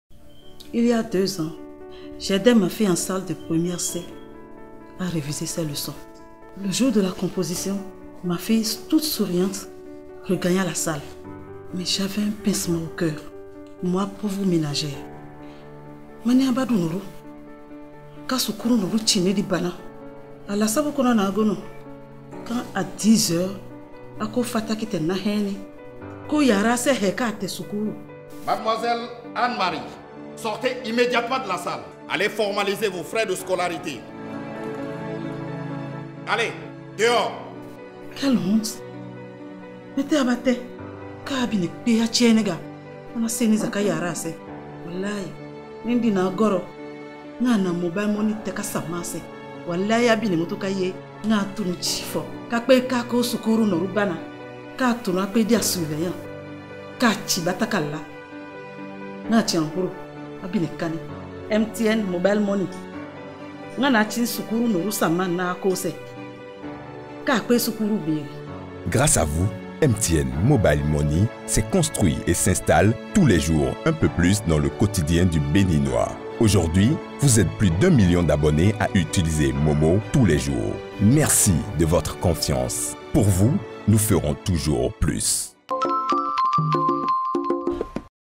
MTN Mobile Money - Pour payer votre scolarité
Pour vous, nous avons conçu des solutions sécurisées et innovantes pour faciliter votre vie chaque jour. Et vous avez apprécié.

De Kandi à Cotonou, vous êtes plus d’un million d’abonnés MTN Mobile Money à nous faire confiance.

Grâce à MoMo, vous avez pu soutenir vos proches, faire prospérer votre commerce, gagner du temps… Et cela ne changera pas.

Nous continuerons ensemble avec nos partenaires à vous offrir le meilleur et à garantir la sécurité des fruits de votre labeur, pour vous permettre d’en profiter quand vous le voulez.

Merci pour votre confiance. 😊